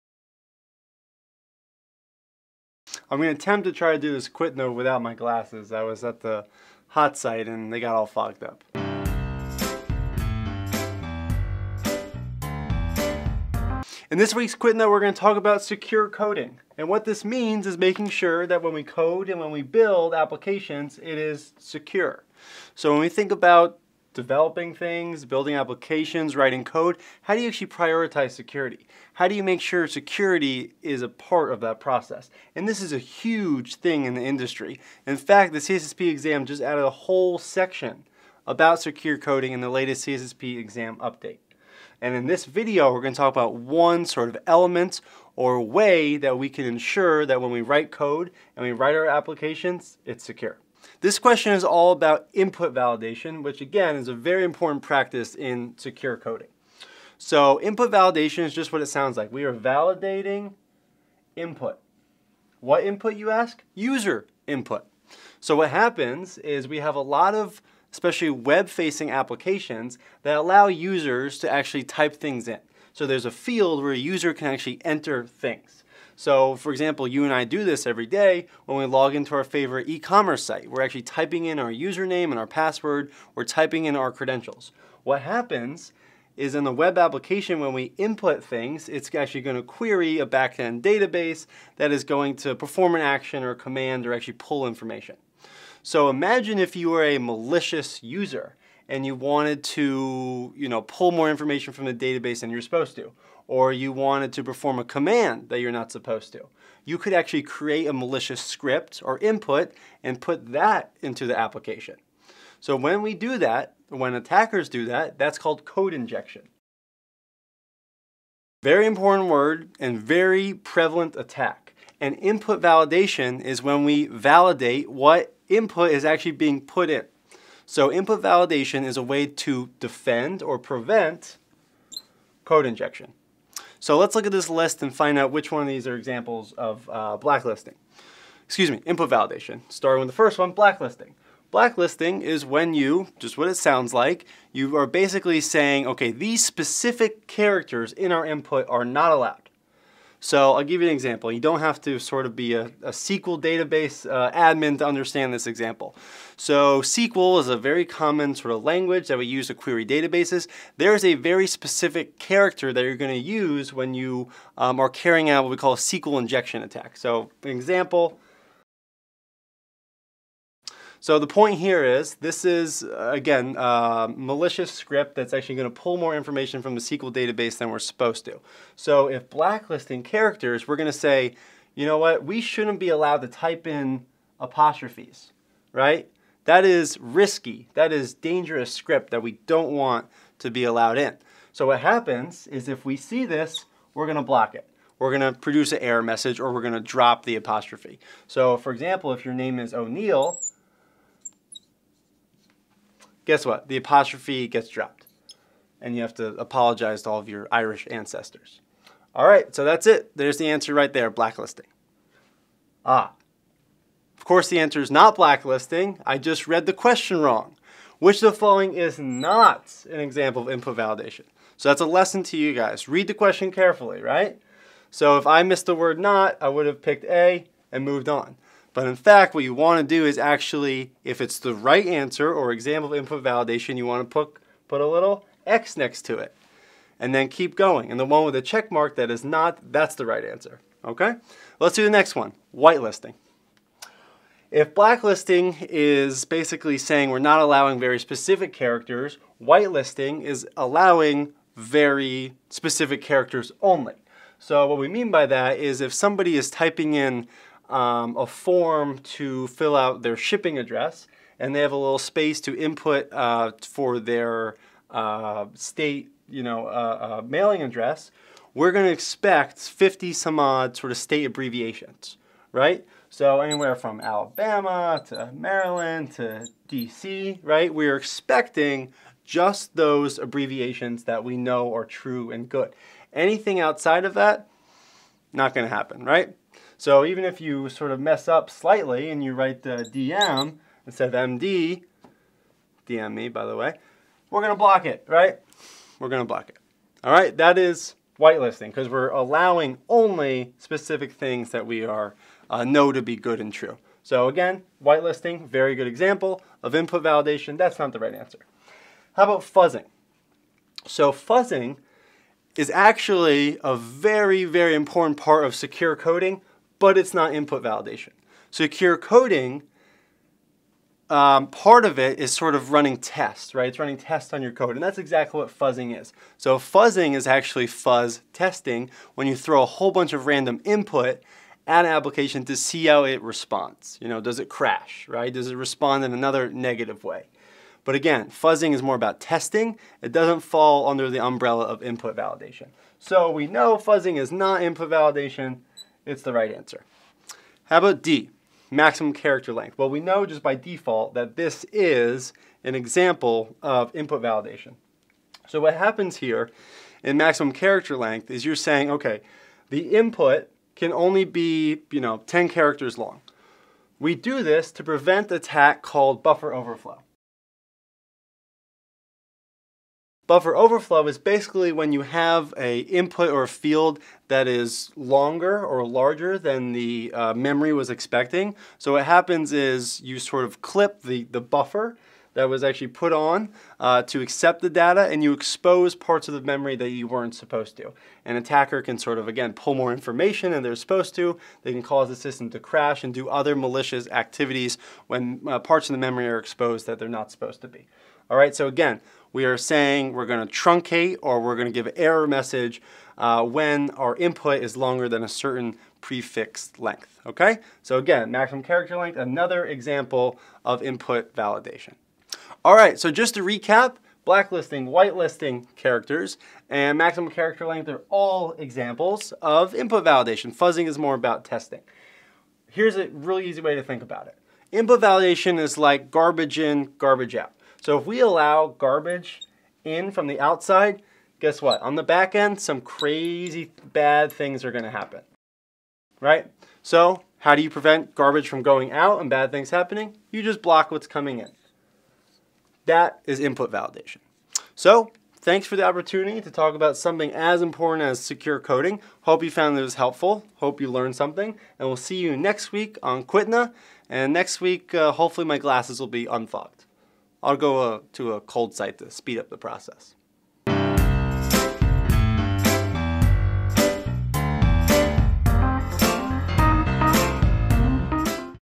QTNA #12 Secure Coding
In CyberVista's Questions That Need Answers (QTNA) video series, we tackle some of the most testable and important cybersecurity questions on a weekly basis. This week we're focused on a topic related to secure code development: input validation.

Which of the following is not an example of input validation?

A. Blacklisting characters
B. Whitelisting characters
C. Fuzzing
D. Maximum character length